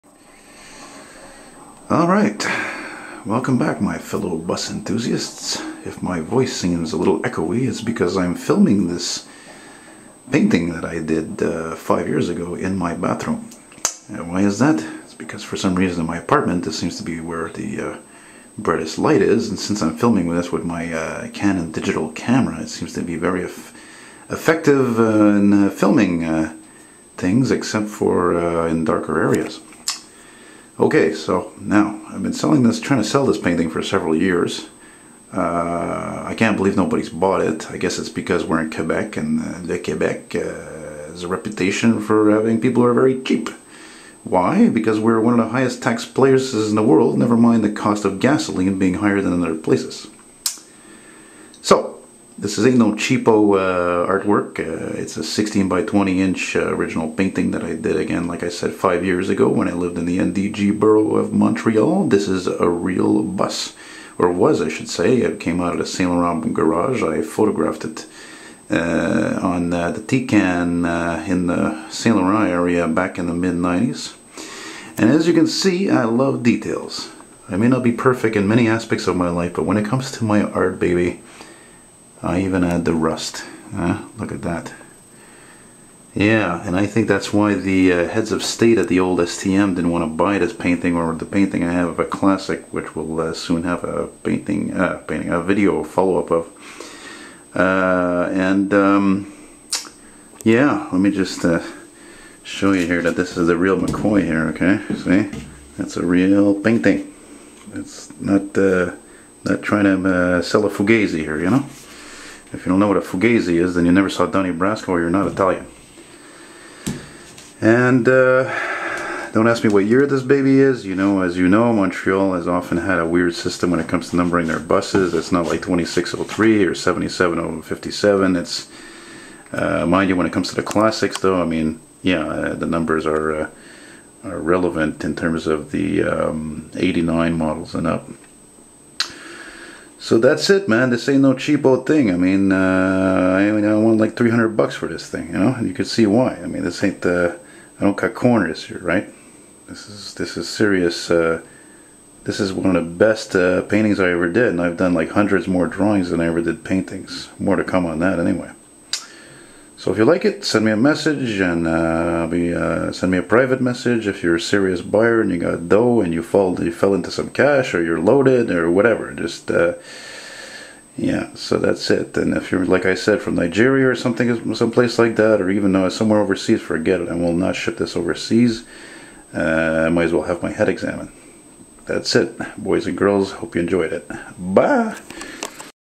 Montreal STCUM Bus Painting FOR SALE GMC New look 24-031
A very realistic painting that I did of an aging fishbowl that I photographed in the mid-Nineties. You should also appreciate that Im visually impaired a cant even drive legally - Elton McFall.  NON-NEGOTIABLE. Bilingue
 I forgot to mention that I think the reason why I couldn't sell this to the STM was because all the people I met there were women. Only guys seem to really be into old buses. Chicks, prove me wrong!